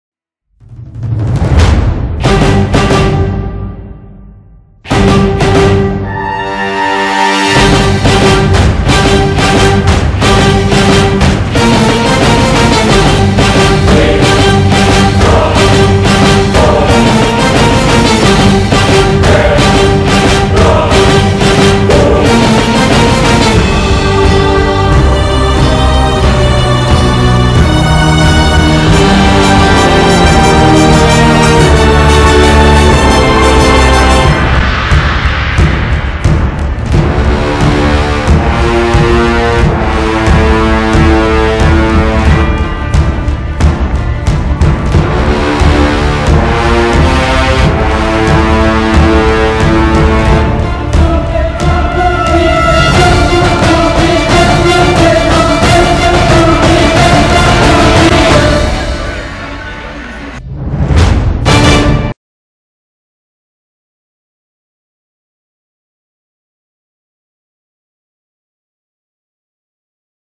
Behind the Scenes with David Beckham
Meet David Beckham practicing with his teammates un preparation for the game between LA Galaxy and Houston Dynamo.  SLIDESHOW